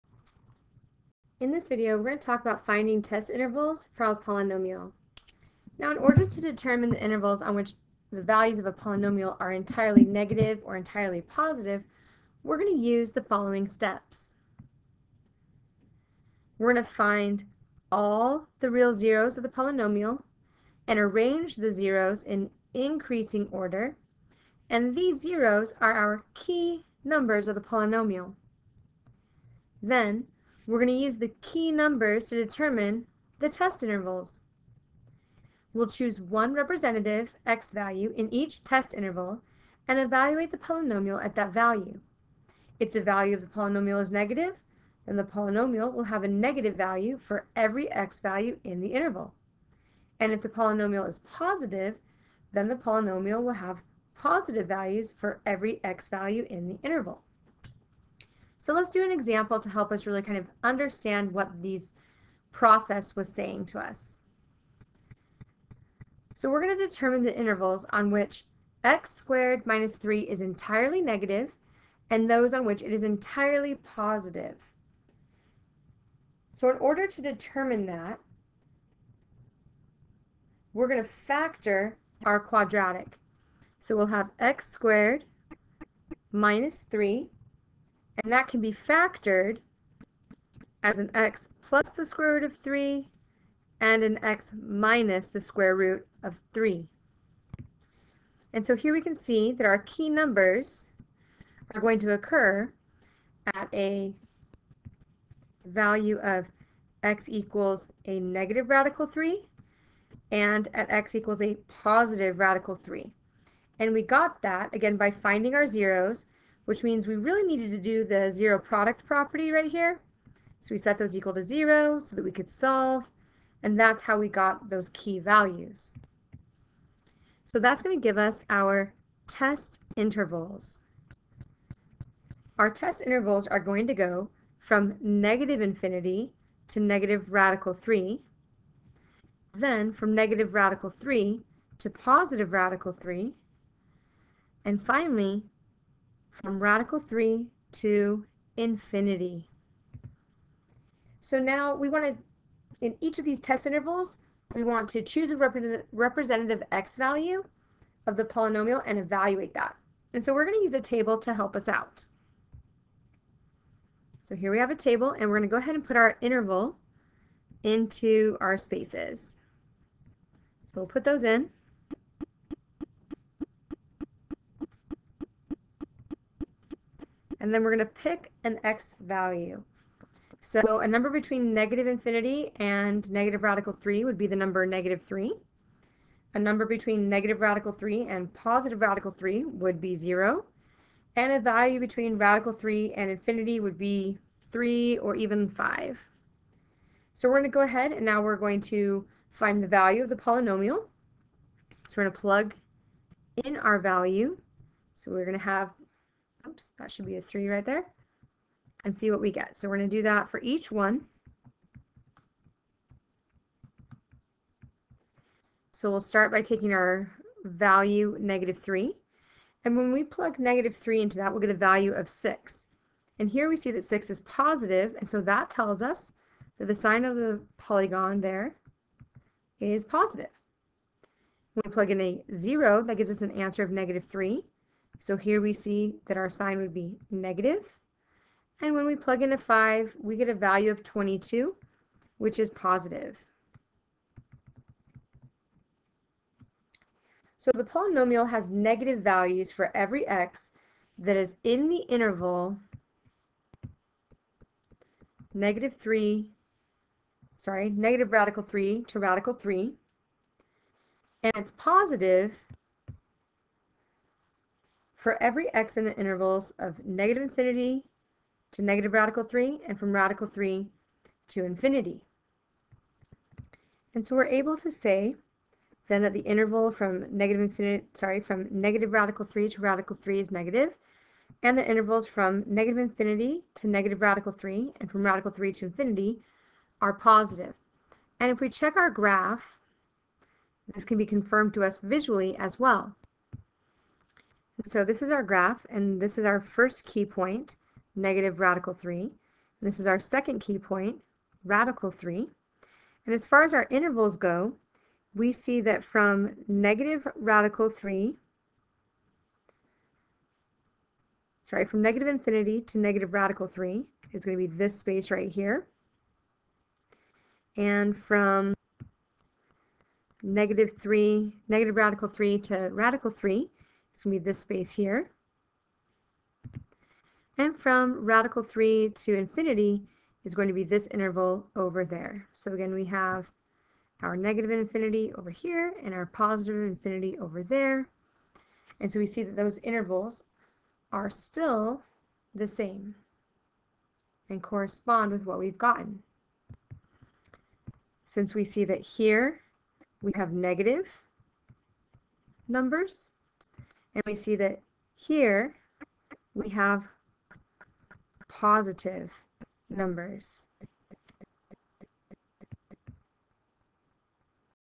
Finding Test Intervals for a Polynomial Video
This video goes through an example of finding test intervals for a polynomial equation and how to determine on which intervals the expression is entirely positive and the interval that is entirely negative.